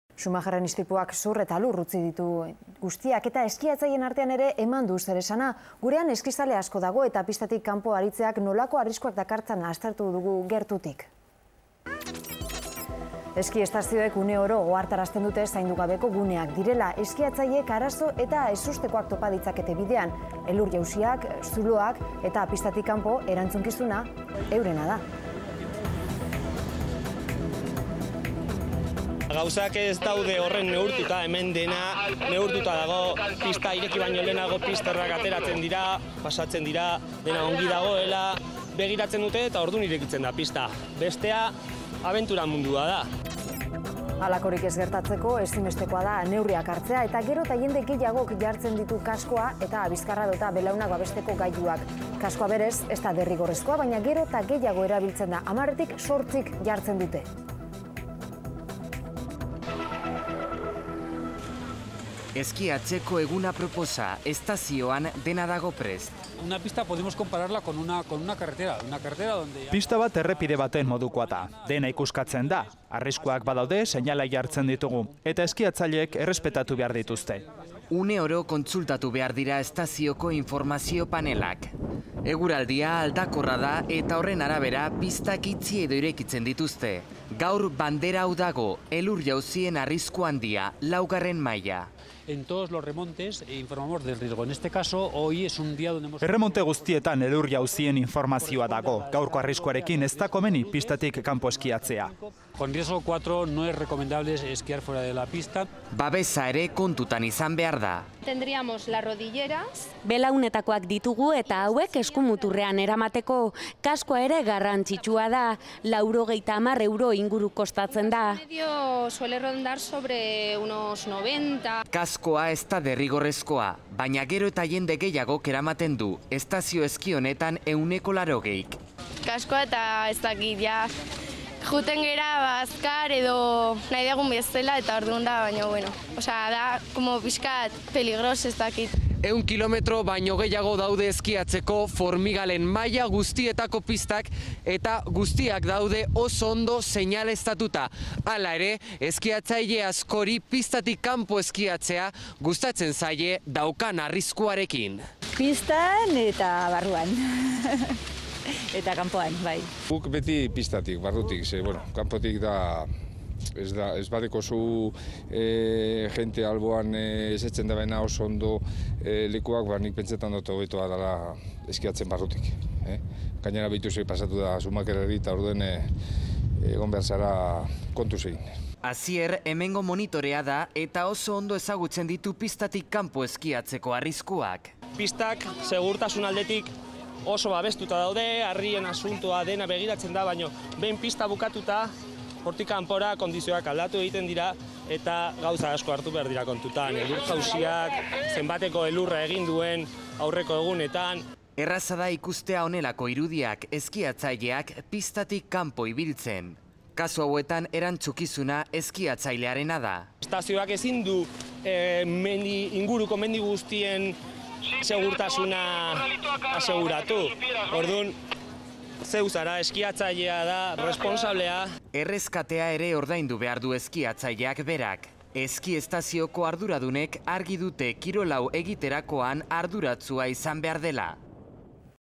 Arriskutsua da pistatik kanpo eskiatzea?
GERTUTIK ezagutuko dugu eski estazioetako segurtasuna eta ze arrisku daukan pistatik kanpo eskiatzeak. Kasu horietan, eskiatzaileak du erantzukizuna.